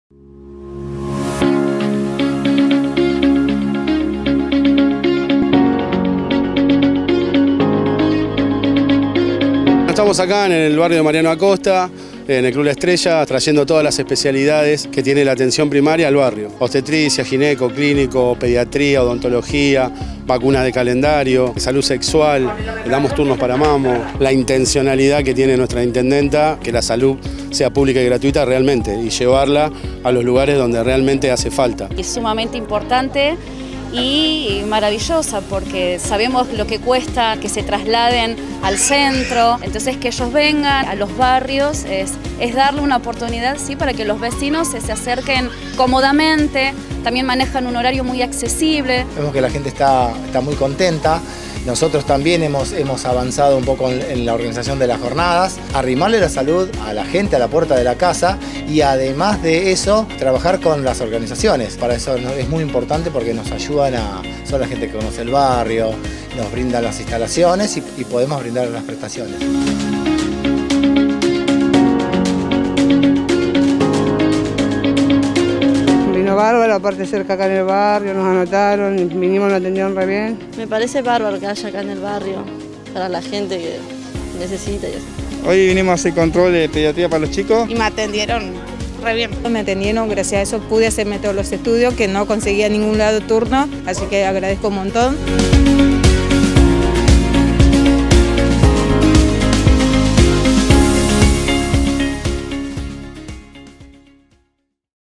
Jornada de salud integral en Mariano Acosta
La Subsecretaría de Atención Primaria de Merlo realizó una nueva jornada de salud del Programa “Mujer, ahora te toca a vos” en el Club Deportivo Inti Huasi (Colombres 4133) en Mariano Acosta.